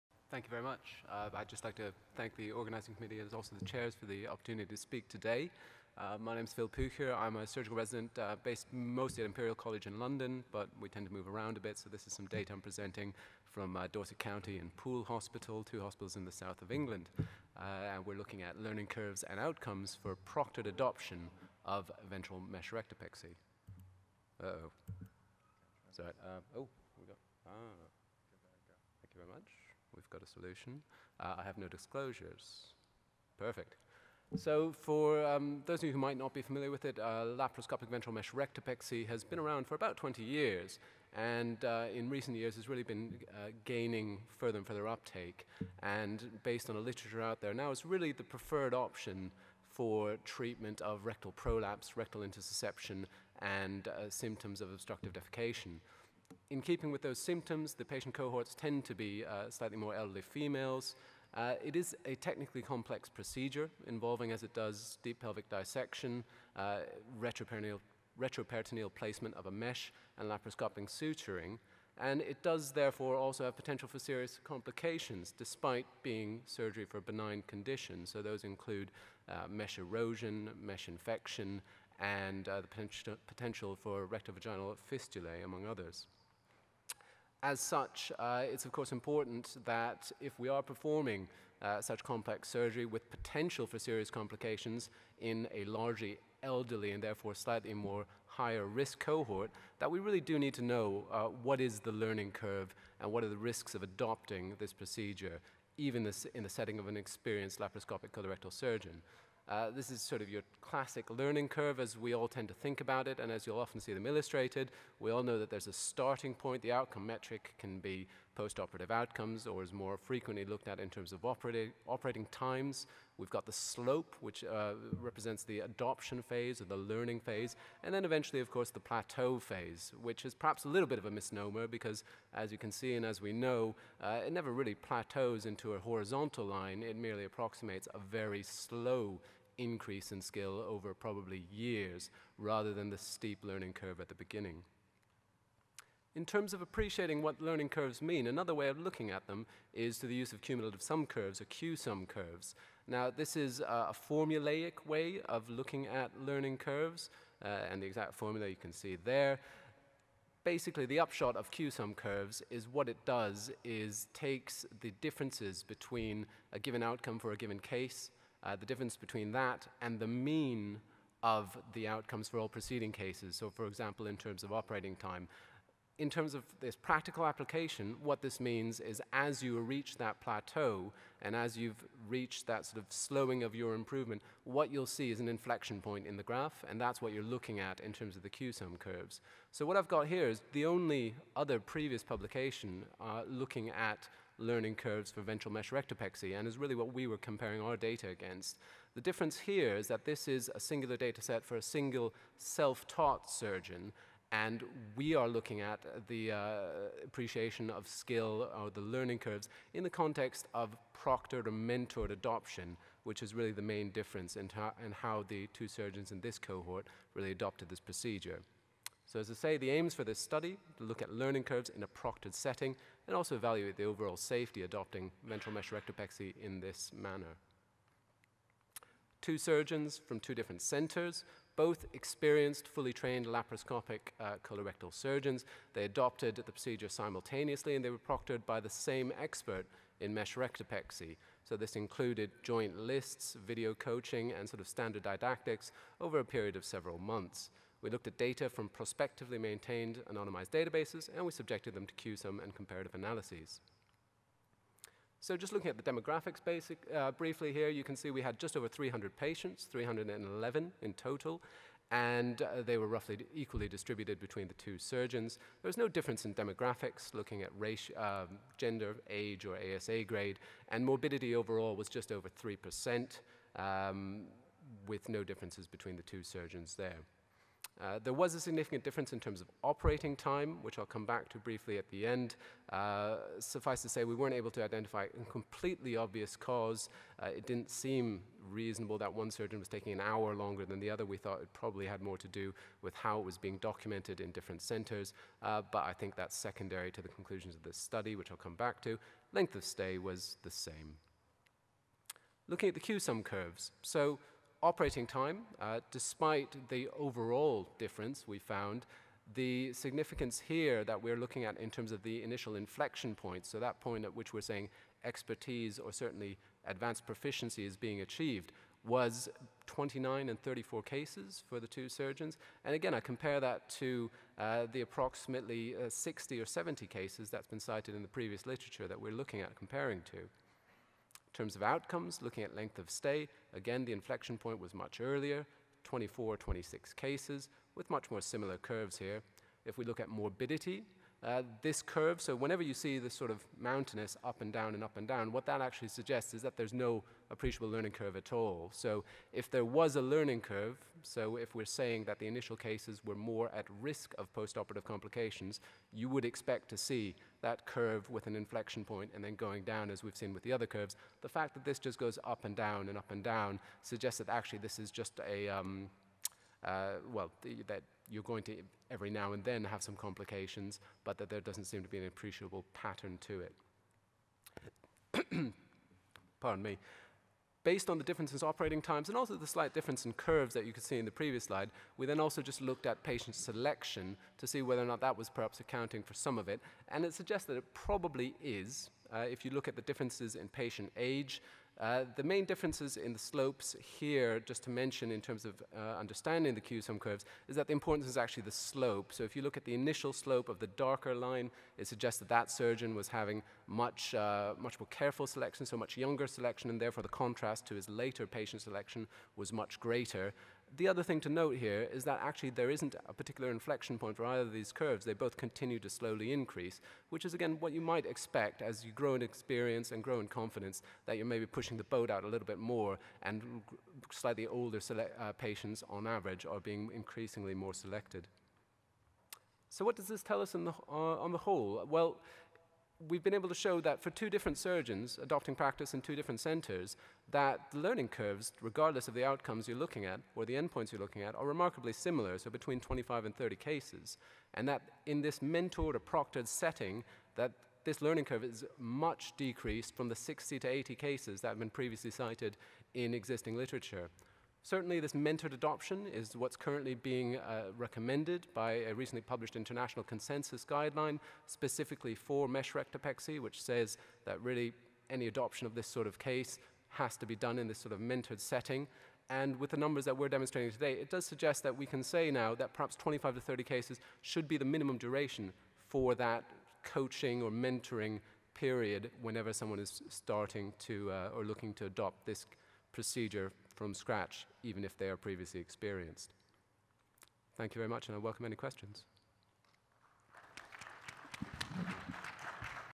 Learning Curves & Surgical Outcomes for Laparoscopic Ventral Mesh Rectopexy: Cumulative Sum Curve
Presented by P. Pucher, MD, PhD MRCS at the SS07: MIS Mixed Bag Session during the SAGES 2016 Scientific Session. March 17, 2016